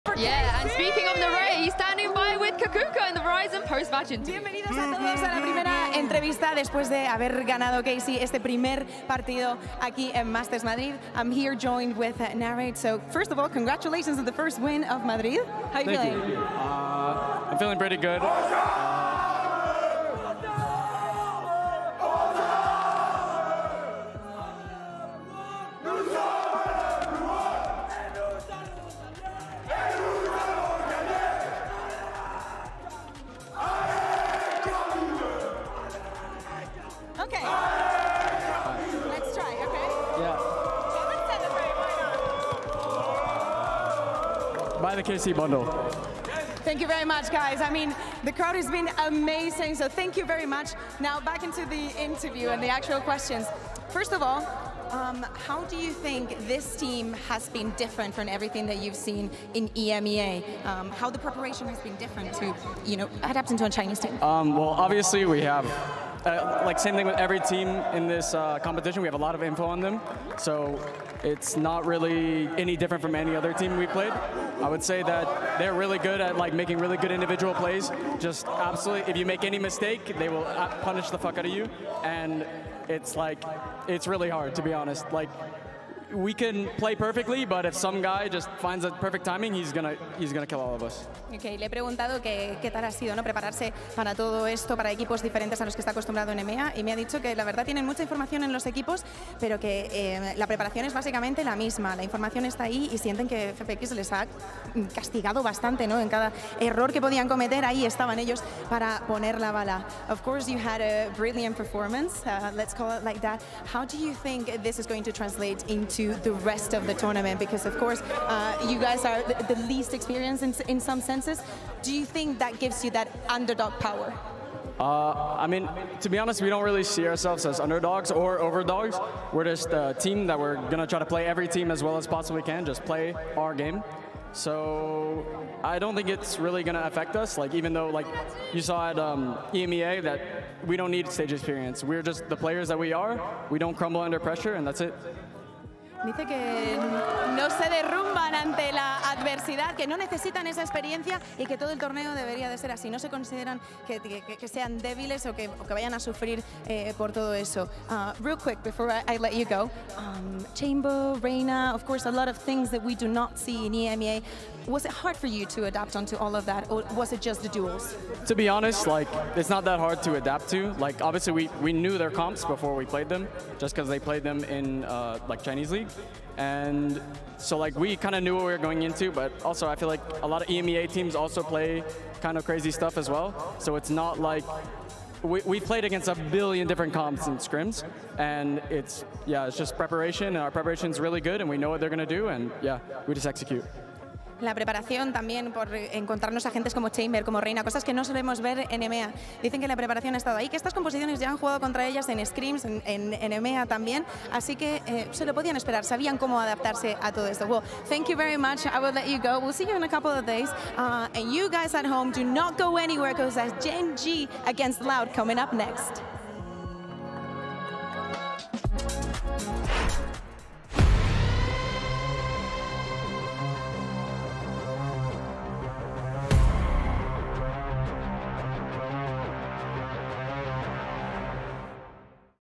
Karmine Corp vs FunPlus Phoenix Post Match Interview with N4RRATE | Valorant Masters Madrid
Here's what N4RRATE has to say about his match against FPX.